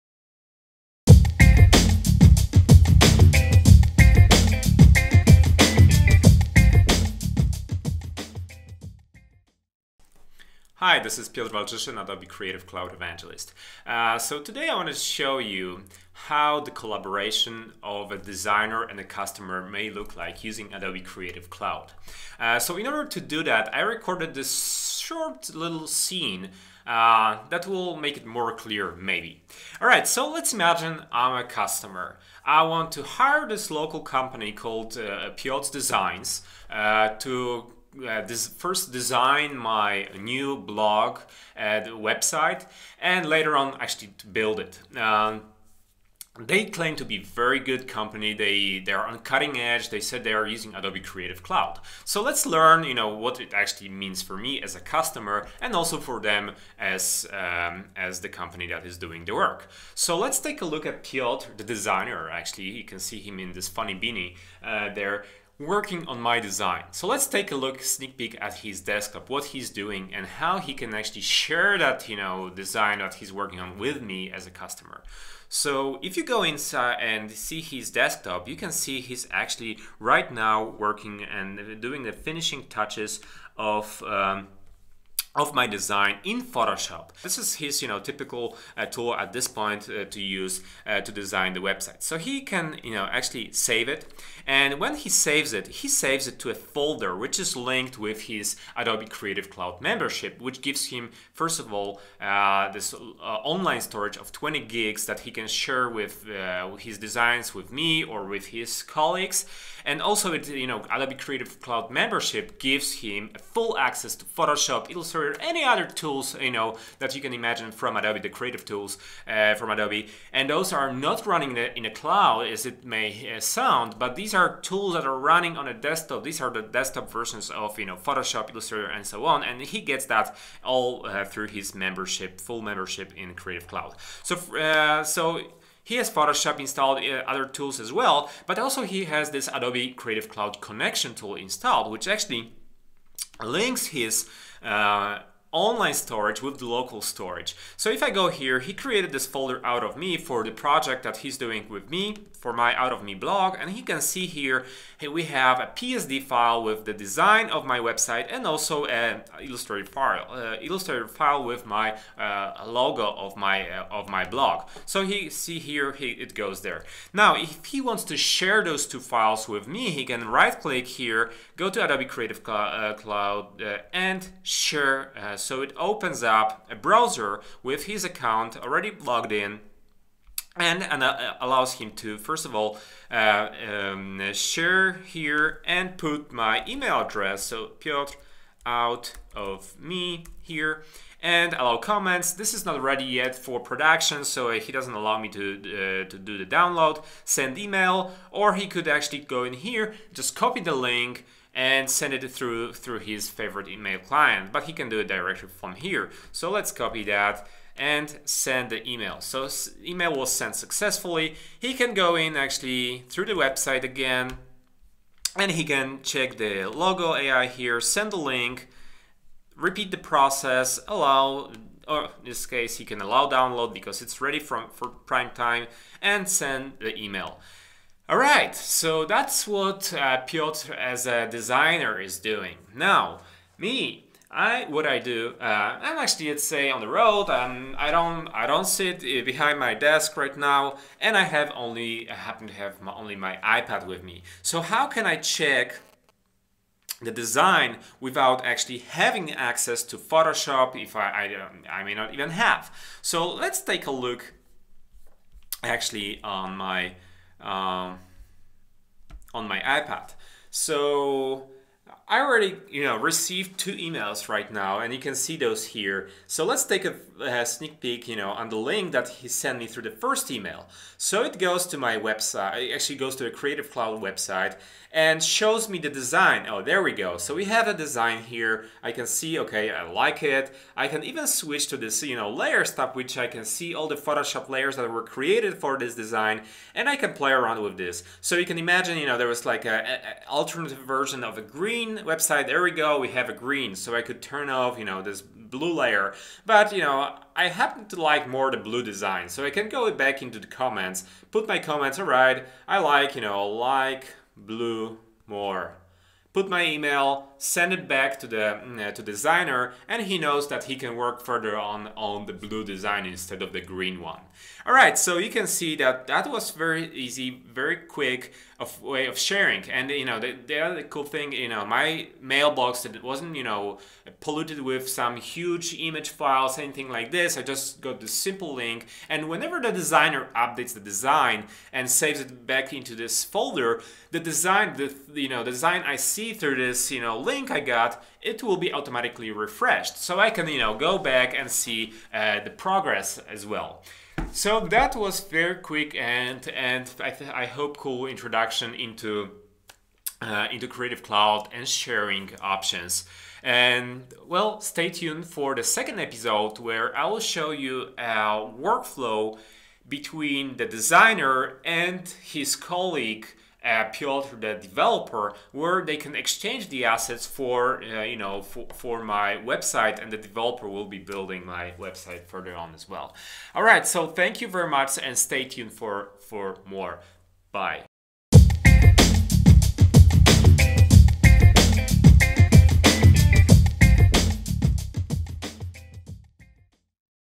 Creative Cloud: Designer - Customer Workflow
In this short video I demonstrate how workflow between designer and customer may look like using Adobe Creative Cloud sharing feature.